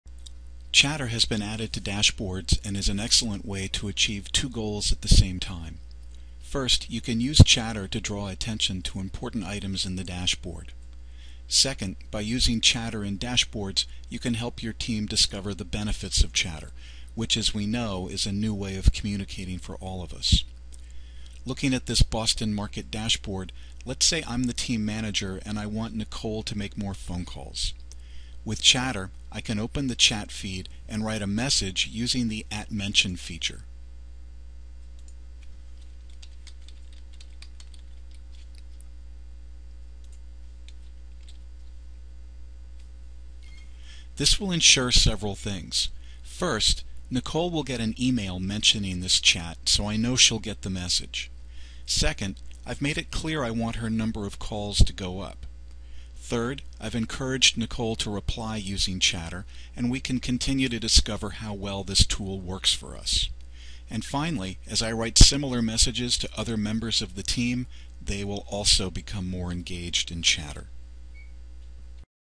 Dashboard Chatter
Learn how to use Chatter on SalesForce dashboards.